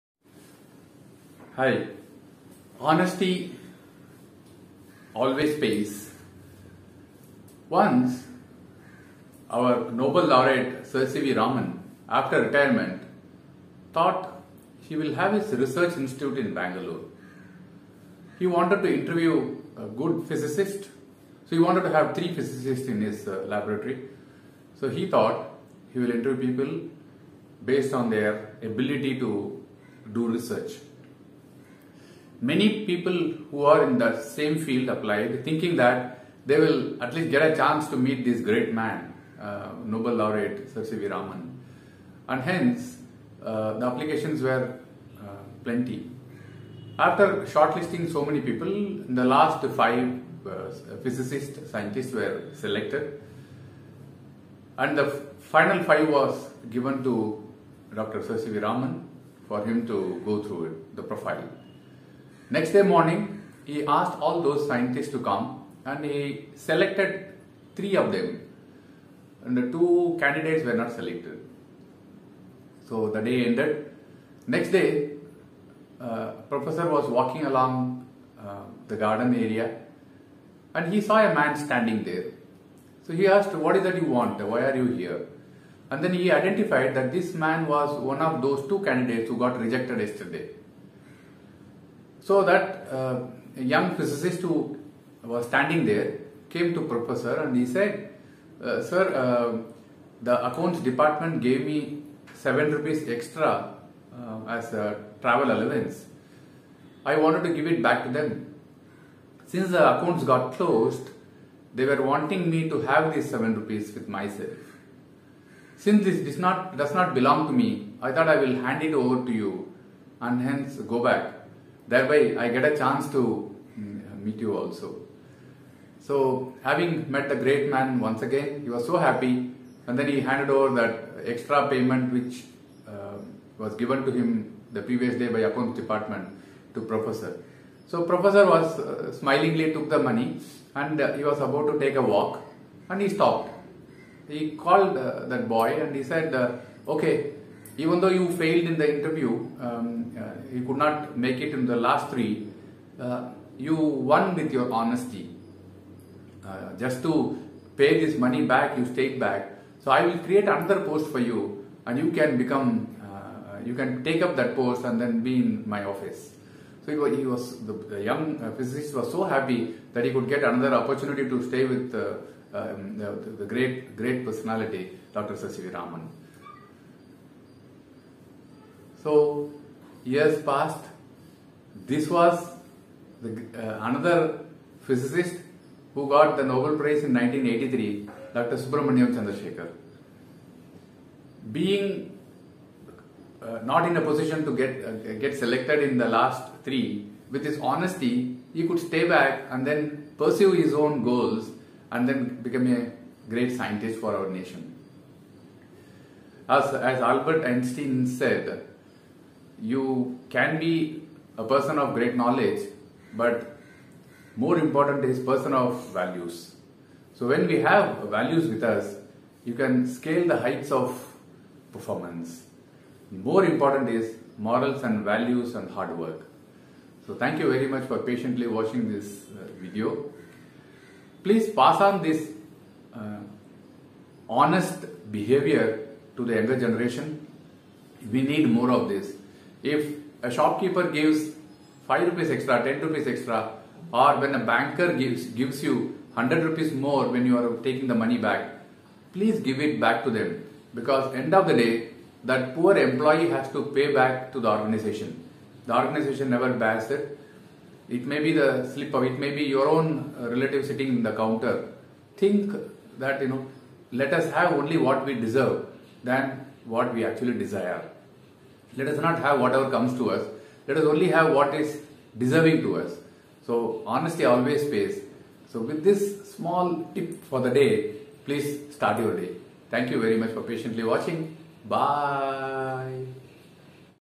Honesty is the best policy | Sir C.V. Raman | 7 rupees that changed my life S Chandrashekhar | story
*₹7 !*📚📚📚

The Nobel Laureate Prof. C. V. Raman after retirement wished to open a Research Institute in Bangalore.

So he gave an advertisement in the newspapers for recruiting three physicists.

Lots of eager Scientists applied thinking that even if they were not selected, they would at least get an opportunity to meet the Nobel Laureate.

In the preliminary selection, five candidates were selected and the final interview was to be taken by Prof. C V Raman himself.

Three were selected out of the five.

Next day Prof. Raman was taking a walk and found one young man waiting to meet him.

He realized that it was the same man who was not selected.

The Prof.  asked him what was the problem and he replied that there was no problem at all, but after finishing the interview the office had paid him ₹7 extra than his claim and he wanted to return it.

But because the accounts had closed, they could not take back the amount and asked him to enjoy.

The man said that it is not right for him to accept the money which did not belong to him.

Prof. C V Raman told him, so you wish to return the ₹7 and he took the money from him.

After going few  steps forward the Prof. asked the young man to meet him the next day at 10.30 am. The man was happy that he would get an opportunity to meet the great man again.

When he met the Prof.  next day the Nobel Laureate told the young man "Son, you failed in the Physics test but you have passed the honesty test. So I have created another post for you".

The young man was surprised and very happy to join.

Later on he too became a Nobel Laureate in 1983. This young man was *Prof. Subrahmanyan Chandrashekhar*

He has written a book on how the seven rupees changed his life.

*This was how Honesty made a Great Scientist*.

What is lacking in Talent can most often be made up for, with Hard work, guidance and help from others, BUT, what is lacking in Character and Values can’t be made up for with anything Ever.

Which is why Einstein said, *“Don’t try to be a person of Success, but always be a Person of Value”*.

Eventually, patience, persistence determination and discipline always win.